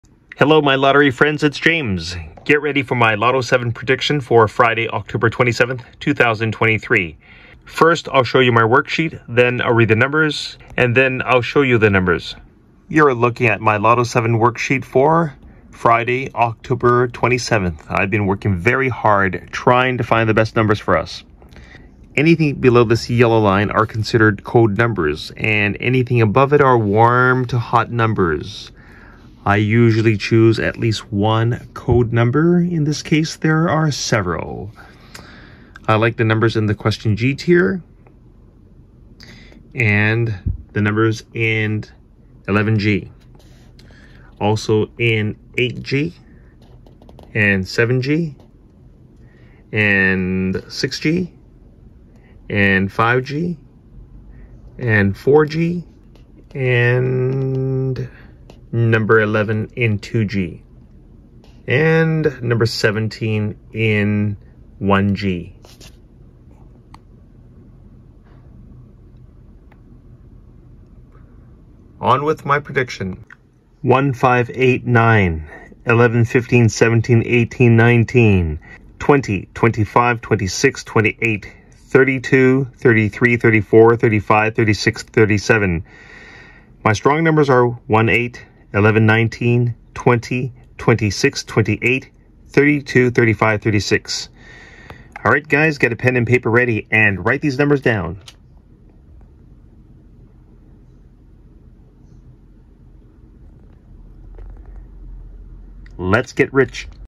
My ロト7 prediction for Fri. Oct. 27, 2023 -My prediction only captured 4/7 of the main numbers
Update!
I am trying 23, 24,29 as well